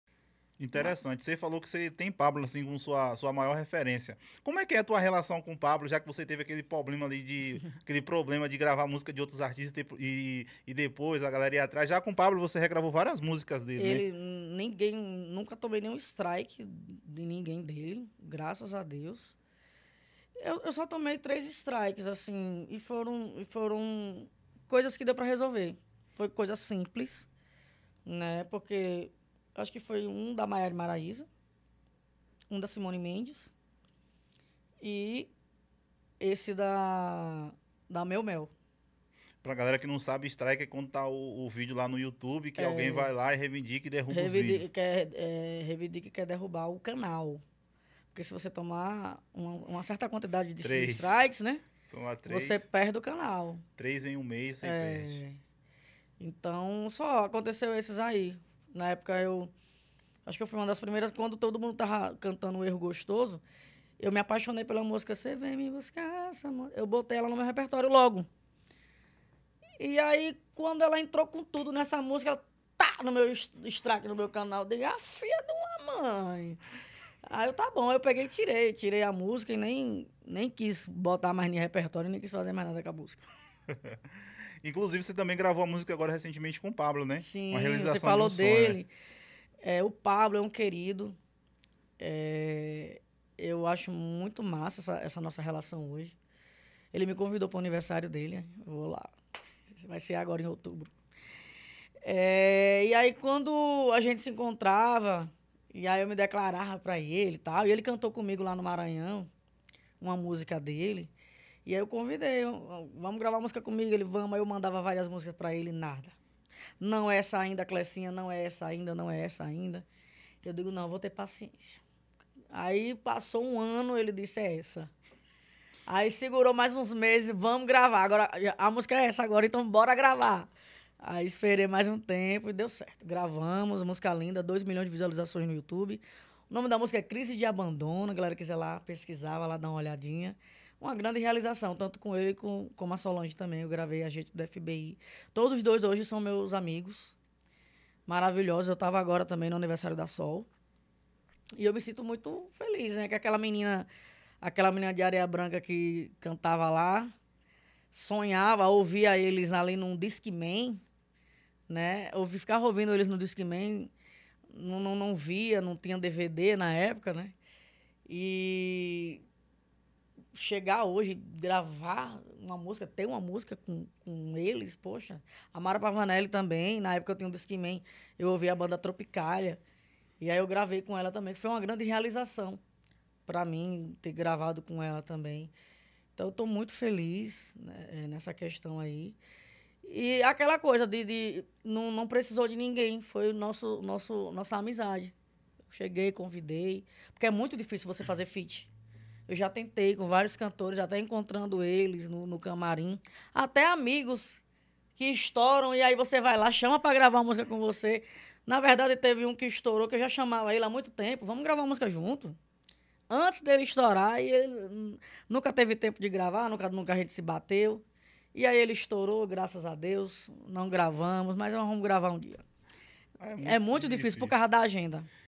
KLESSINHA ABRE O JOGO E FALA COMO É SUA RELAÇÃO COM PABLO
KLESSINHA FALA COMO É SUA RELAÇÃO COM PABLO.